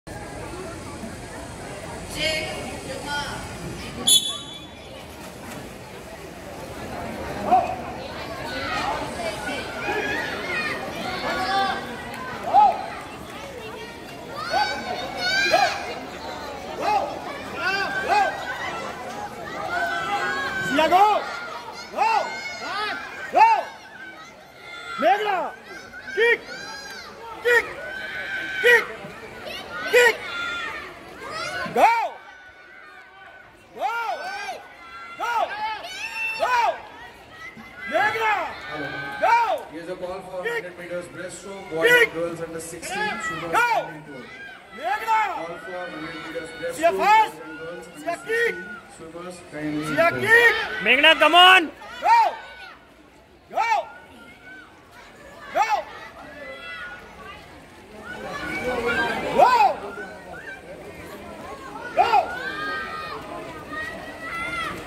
Meghana's Swimming competition -Breaststroke
Breaststroke 50 meters by Meghana. Lane 2 from right. Pink cap.